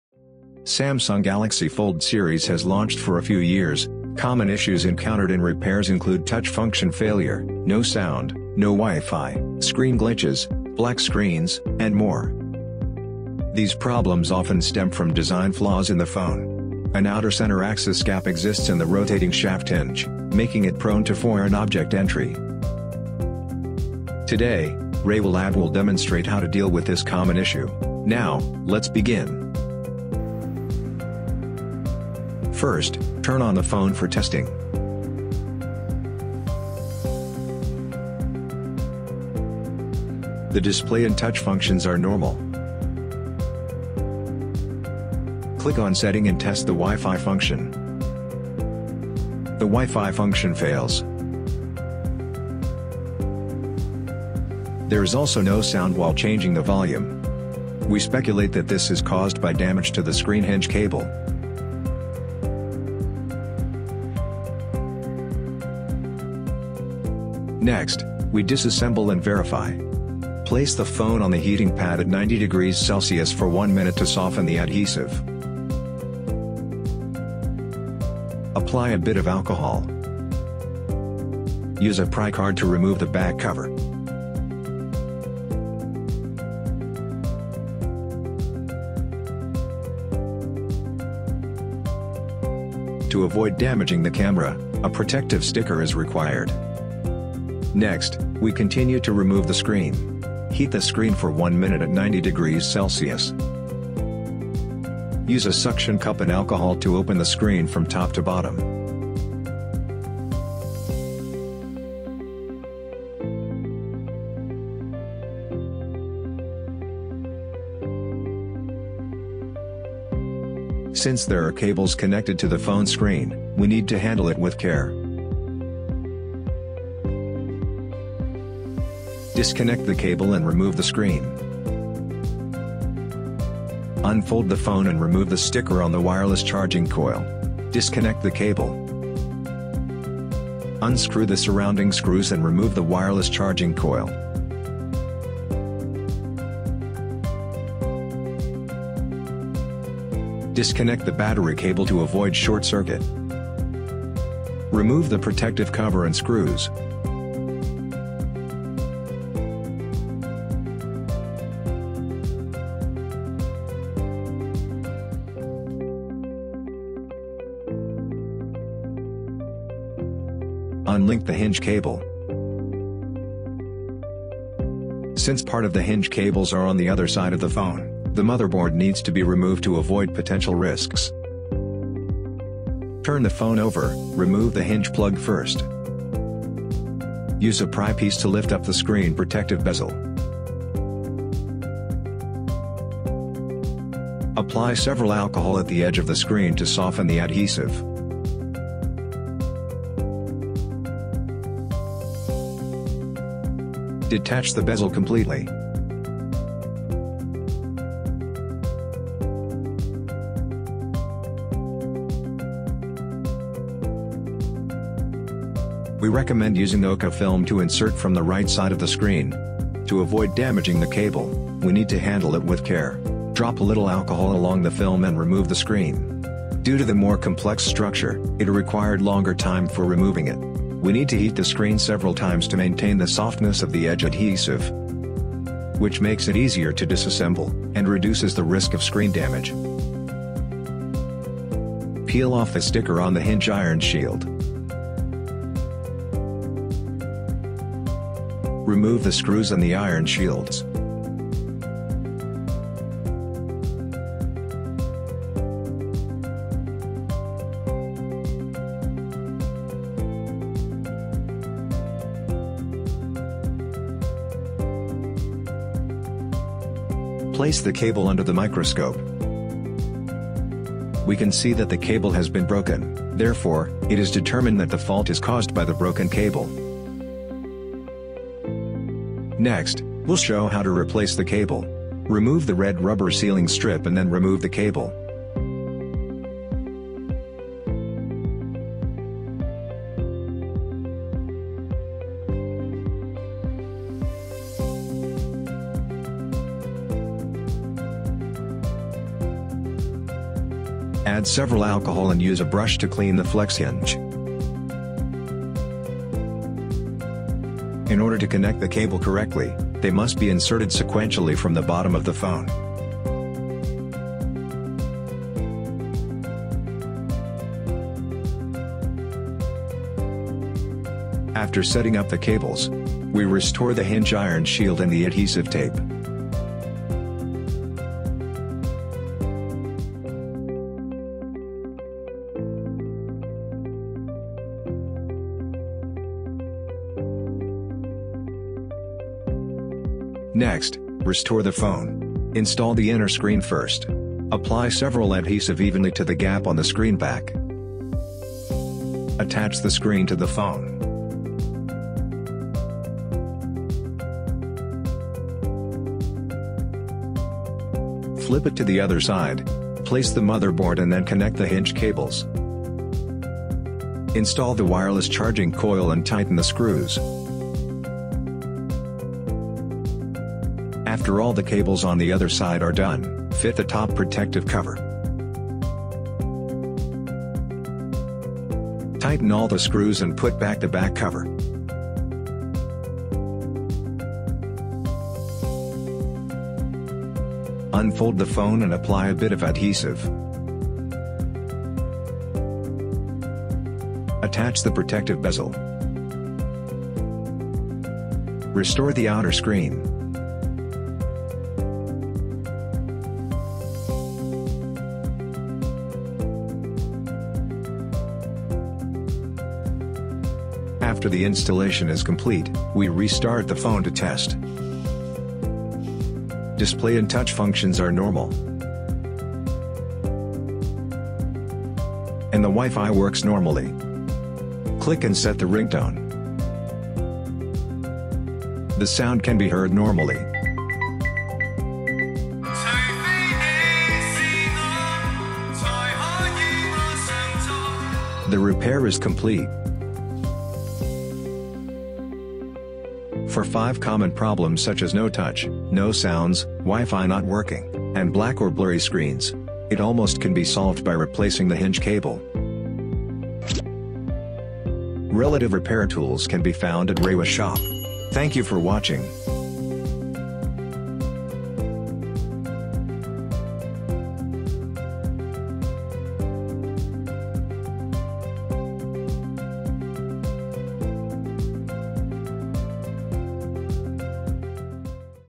Samsung Z Fold 3 Hinge Flex Cable Replacement
Samsung released the Z Fold series a few years ago. However, the phone will have some common faults including "Black Screen" "Abnormal Touch" etc. In today's case, REWA LAB fully disassembles a Z Fold 3 which has "No Sound" and "Wi-Fi Grey Out" issues. We successfully fixed it by replacing the hinge flex cable. We believe these kinds of issues are mainly caused by the flex cable breakage. So what are the tips that you need to notice, you are in the right place.

Repair Tools

REFOX RS3 3D Screwdriver Set for Phone Disassembly Repair

REFOX RS40 Magnetic Screwdriver Holder for Screwdrivers/Tweezers/Pry Bars Storage